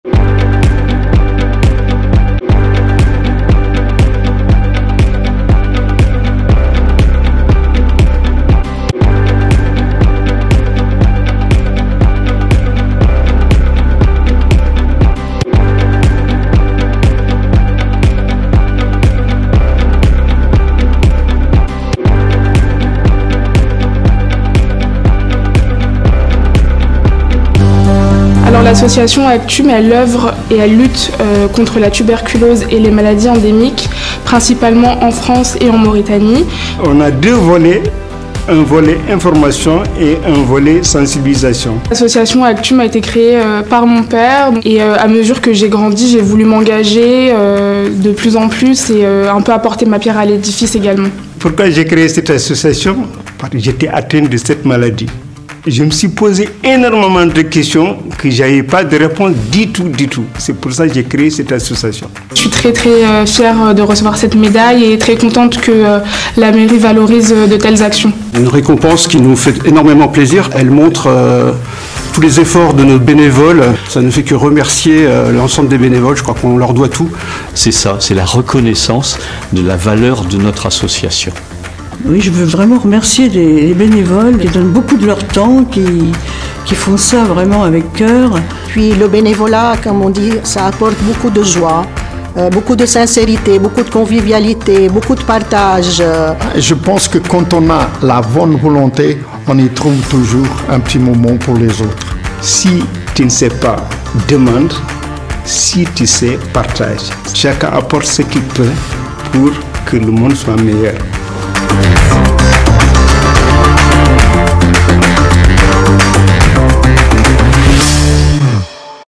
Journée Mondiale du Bénévolat 2023 : ACTUME doublement récompensée par la Mairie de Courbevoie
ACTUME a été honorée par la mairie de Courbevoie lors de la Journée Mondiale du Bénévolat le 5 décembre 2023, remportant deux prestigieux prix : Médaille d’or dans la catégorie Association et dans la catégorie Jeune Bénévole.
La cérémonie de remise des trophées a été présidée par Monsieur le maire de Courbevoie, Monsieur Jacques Kossowski, et a compté sur la participation de Madame Catherine Morelle, Adjointe au Maire déléguée à la vie associative.